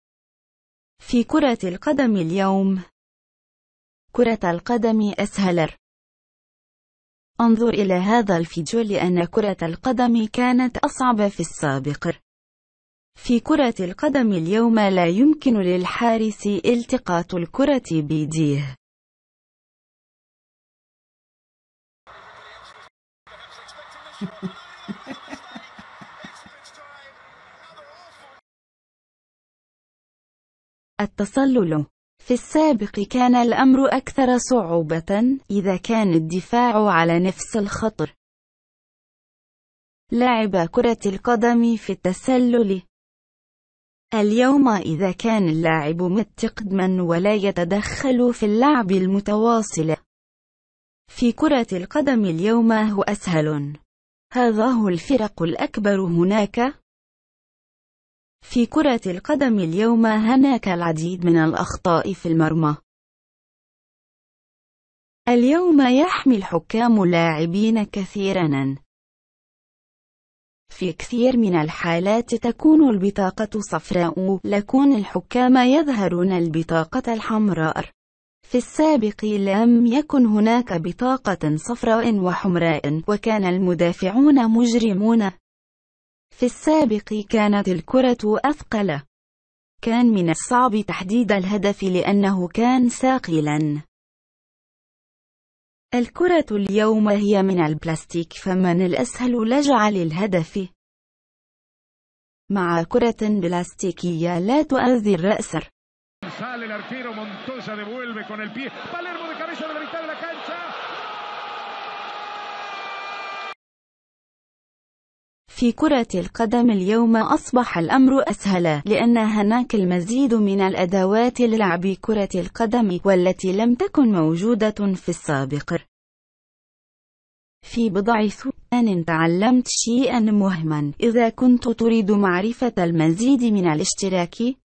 1960-1970 كانت كرة القدم أصعب من كرة القدم اليوم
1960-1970 كانت كرة القدم أصعب من كرة القدم اليوم